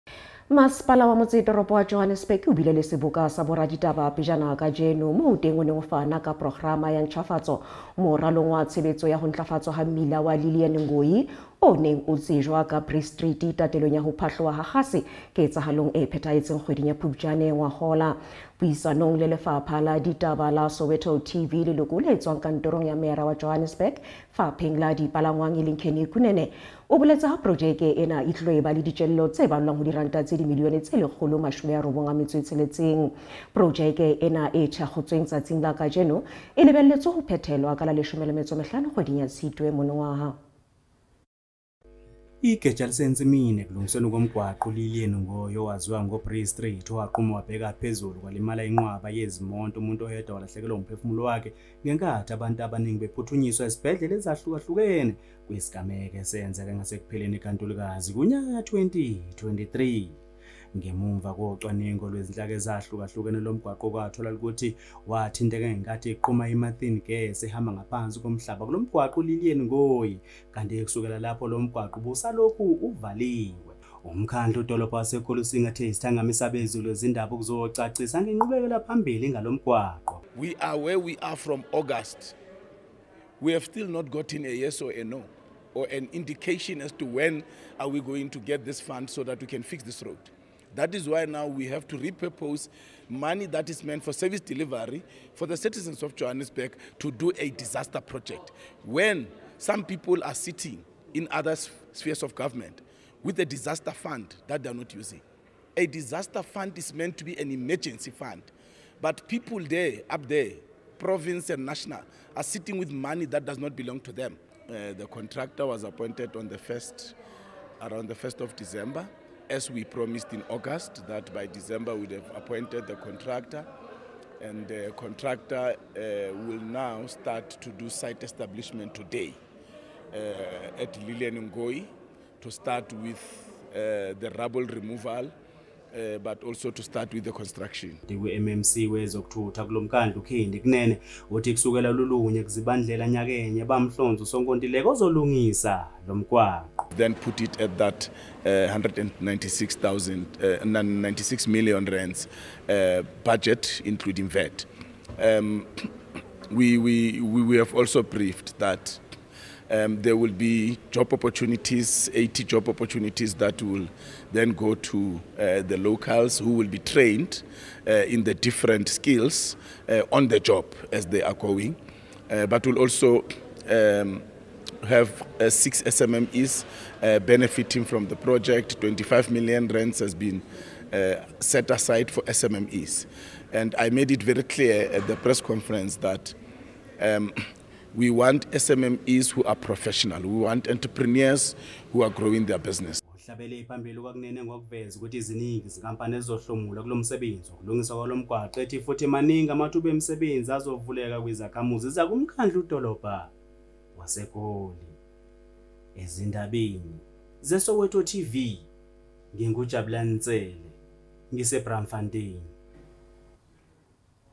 SOWETO TV NEWS | PROGRAMA YA HO NTLAFATSWA MMILA WA LILLIAN NGOYI HO HLOLA DITJELLO TSA R190M: COJ
Maspala wa Johannesburg o bile le seboka sa boraditaba pejana kajeno, moo teng o neng o fana ka programa ya ntjhafatso moralong wa tshebetso ya ho ntlafatswa ha mmila wa Lillian Ngoyi o neng o tsejwa ka Bree Street, tatelong ya ho phatloha ha kgase ketsahalong e phethahetseng kgweding ya Phupjane ngwahola.